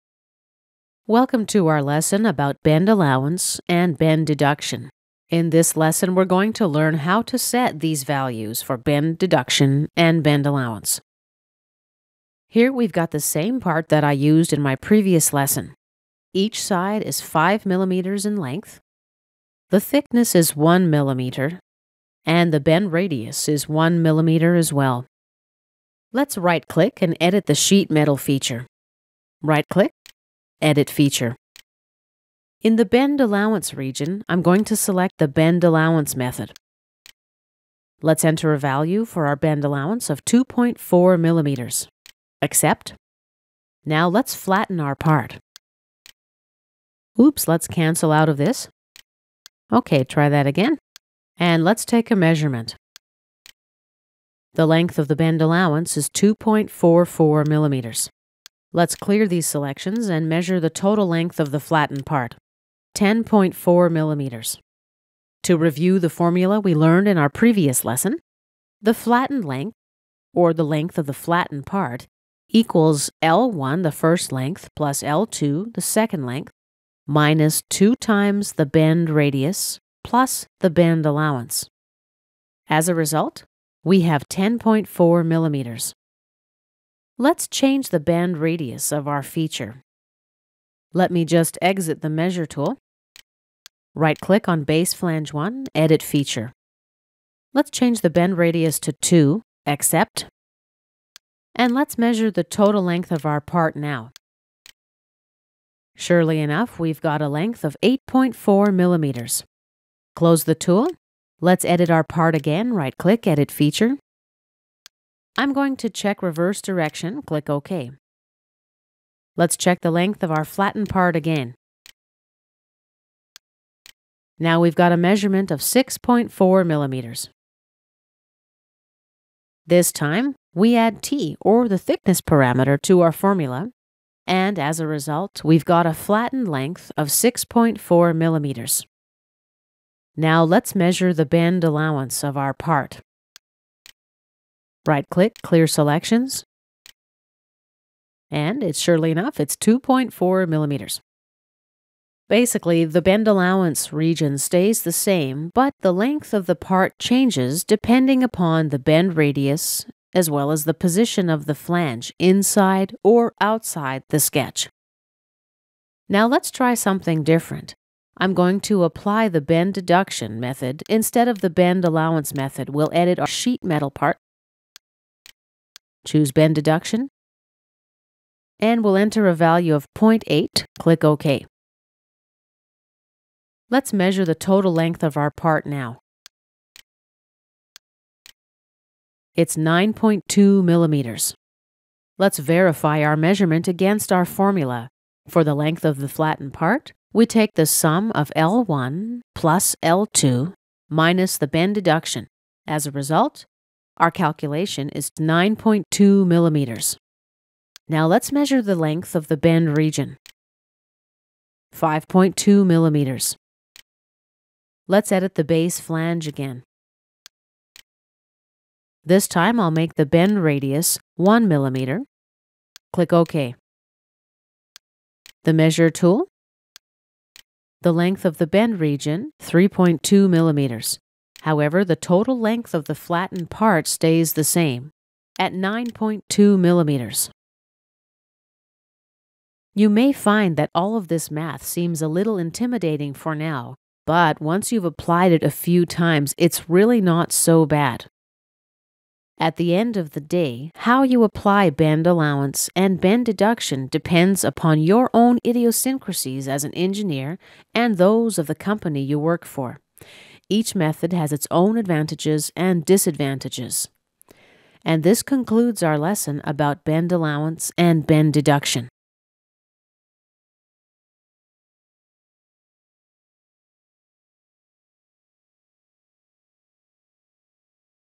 SOLIDWORKS Sheet Metal 04 ( Bend Allowance & Deduction ) Tutorial
video-tutorials.net